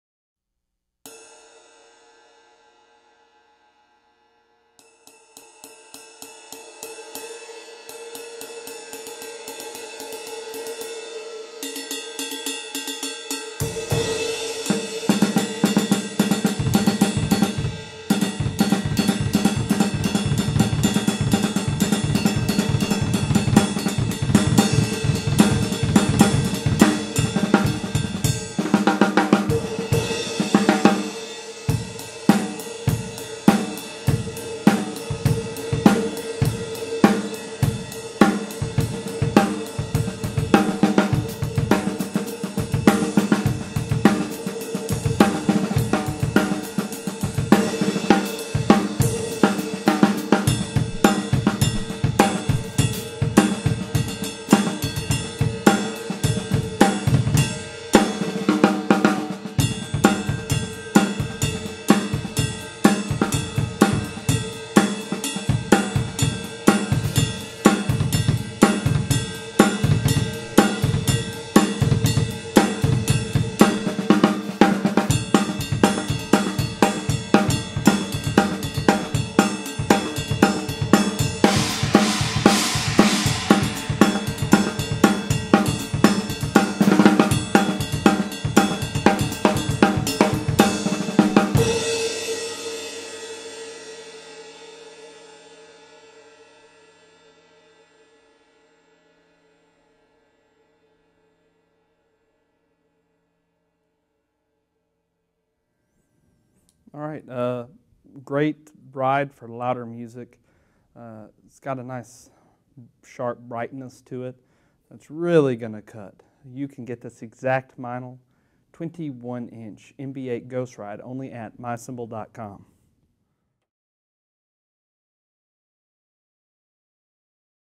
Meinl 21" MB8 Ghost Ride Cymbal (MB8-21GR-1100714Y)
*SOLD* Meinl 21" MB8 Ghost Ride Cymbal available from Memphis Drum Shop.

Other gear used in this video:
• Cymbals (From drummers L to R):  Meinl 14" Byzance Traditional Medium Hi Hat Cymbals, Meinl 20" Byzance Extra Dry China Cymbal